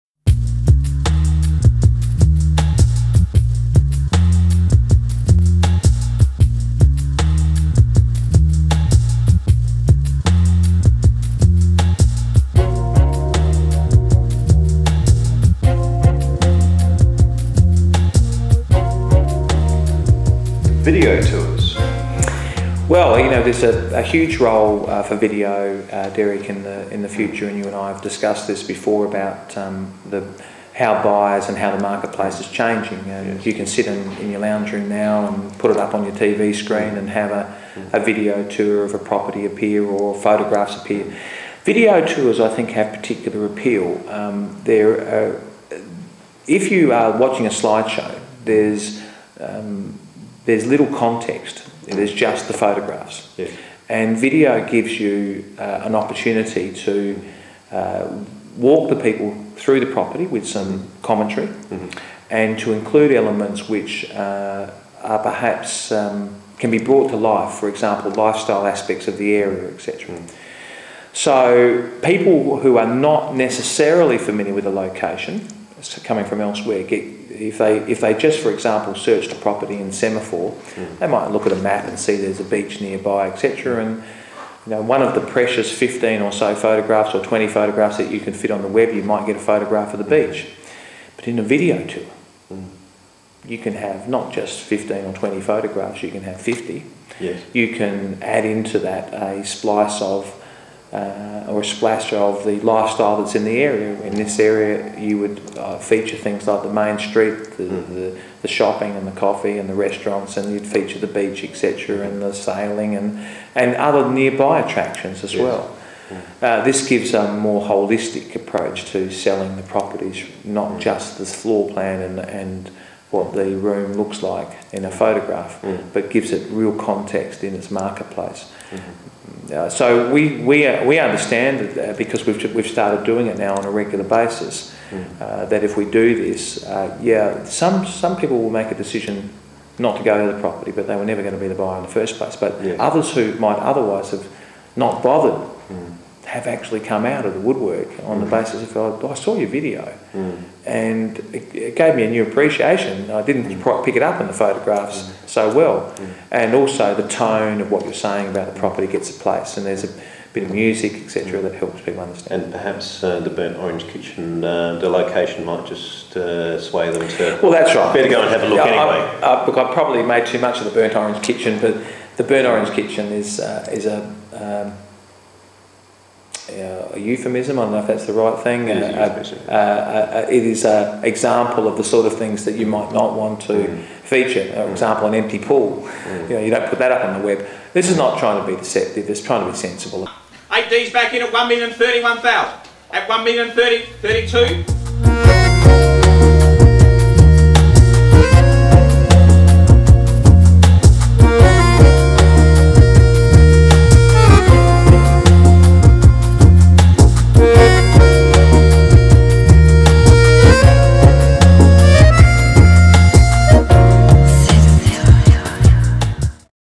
Brock Harcourts Real Estate property video tours
Steve von der Borch on the rise of the online property tour - video, that is. Video showcasing the home and the location.